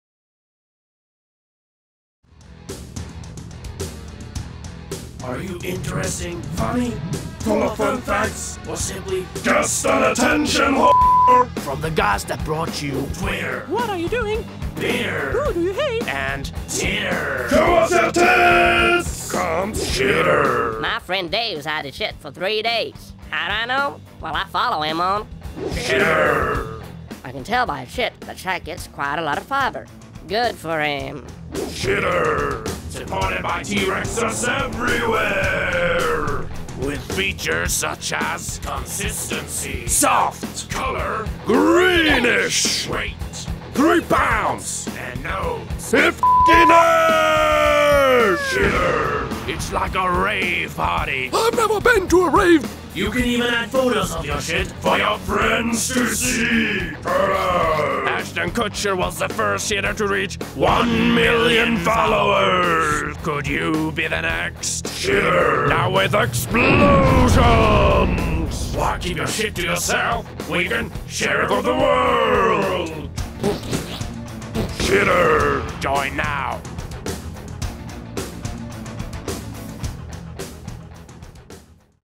Shitter!
Funny Twitter poop spoof!

This is satire, hopefully you'll see it as something more than just fart jokes and foul language:)

We love Twitter. It's a good way of keeping up to date on subjects and people you are interested in. What we don't like about Twitter are all the mindless, no-value tweets.
"I'm going to the bathroom"
"Putting on a sock"
"Itching"

Now that just sounds like shit.
None of the following have viewed or approved this video

Sounds:
Freesound.org

Effect templates by videocopilot.net